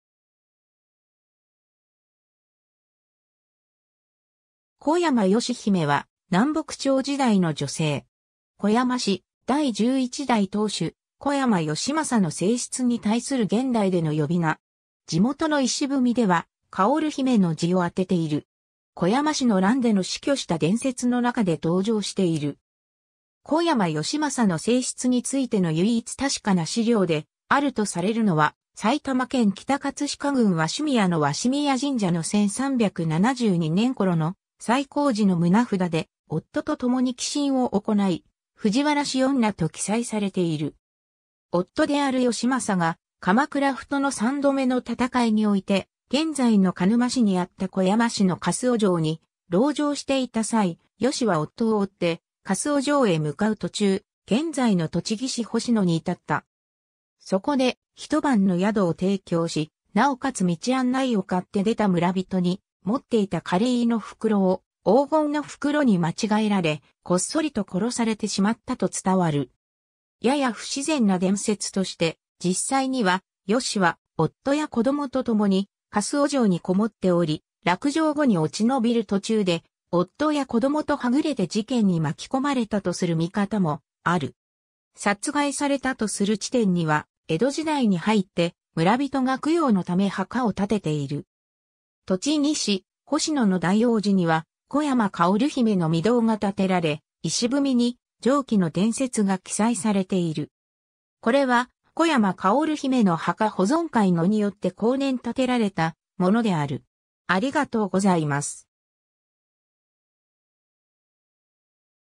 小山よし姫
南北朝時代の女性_(日本)
小山 よし姫（おやま よしひめ）は南北朝時代の女性。
小山氏第11代当主・小山義政の正室に対する現代での呼び名。
地元の碑では芳姫の字をあてている。
小山氏の乱での死去した伝説の中で登場している。
 小山義政の正室についての唯一確かな史料であるとされるのは、埼玉県北葛飾郡鷲宮の鷲宮神社の1372年頃の再興時の棟札で、夫と共に寄進を行い、藤原氏女と記載されている。
 夫である義政が鎌倉府との三度目の戦いにおいて、現在の鹿沼市にあった小山氏の粕尾城に籠城していた際、よしは夫を追って粕尾城へ向かう途中、現在の栃木市星野に至った。
そこで一晩の宿を提供し、なおかつ道案内をかって出た村人に、持っていた乾飯の袋を黄金の袋に間違えられ、こっそりと殺されてしまったと伝わる。
やや不自然な伝説として、実際にはよしは夫や子供とともに粕尾城に籠っており、落城後に落ちのびる途中で夫や子供とはぐれて事件に巻き込まれたとする見方もある。
 殺害されたとする地点（小山城と粕尾城を結ぶ30km程度続く街道のうち、粕尾城まで約8kmの地点、古道の大越路峠、現在は林道）には、江戸時代に入って村人が供養のため墓を建てている。
 栃木市星野の大応寺には「小山芳姫の御堂」が建てられ、碑に上記の伝説が記載されている。
これは「小山芳姫の墓保存会」のによって後年建てられたものである。